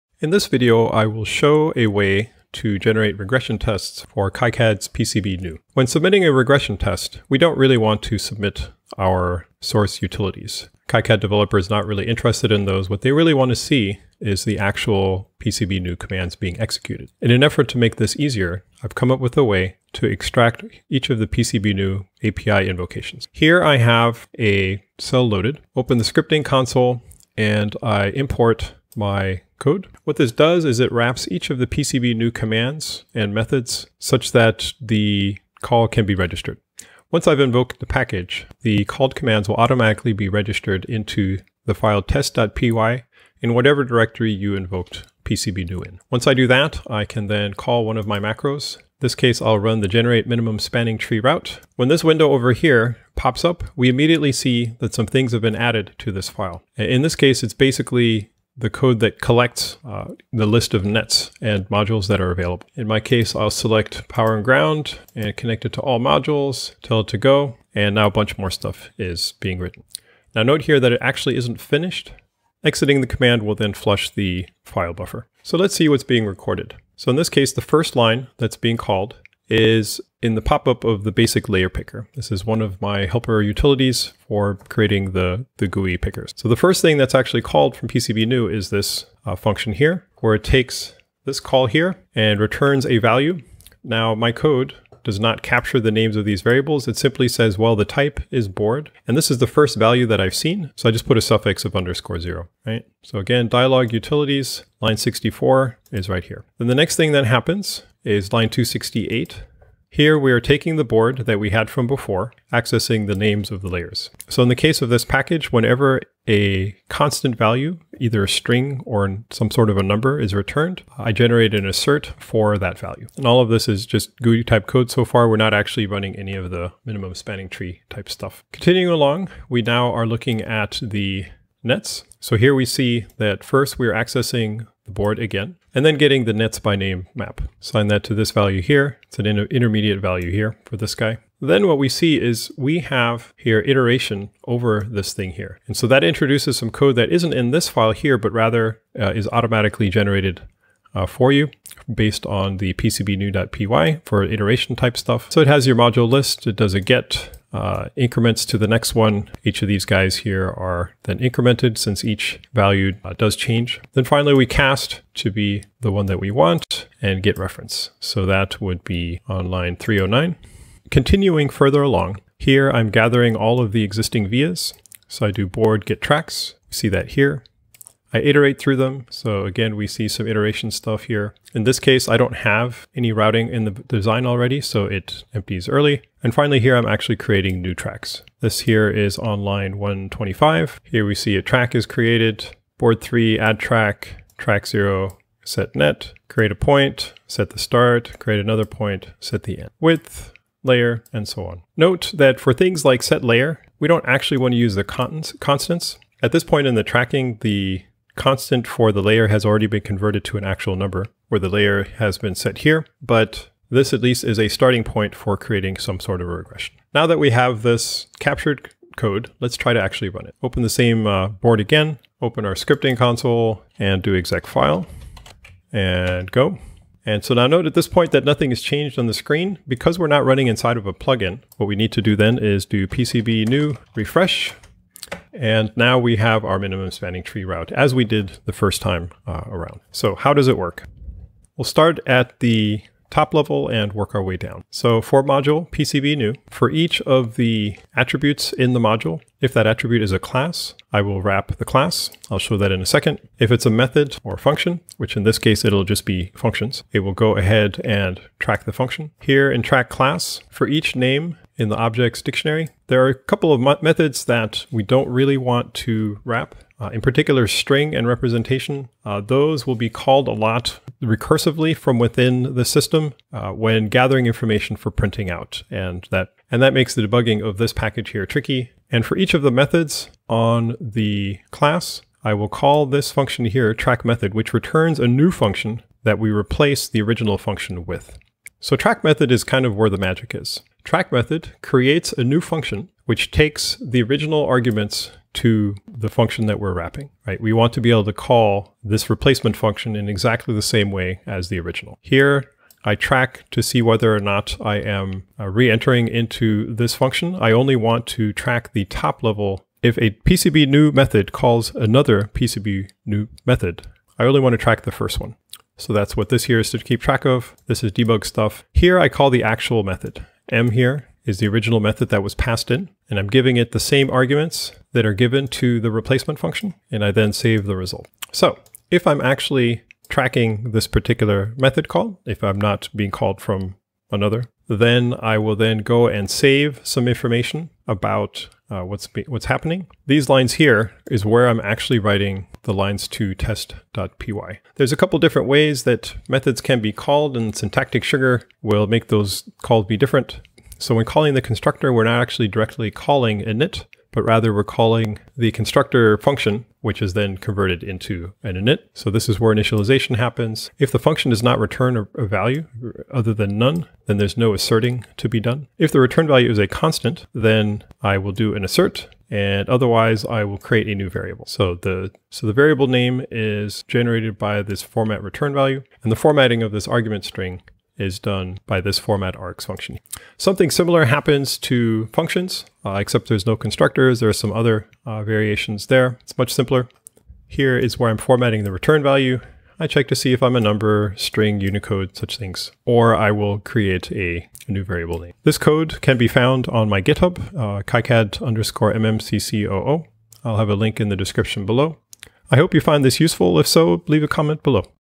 Tracking which pcbnew python commands are being invoked
In this video, I describe a way to generate pcbnew python regressions by recording the commands invoked by plugins.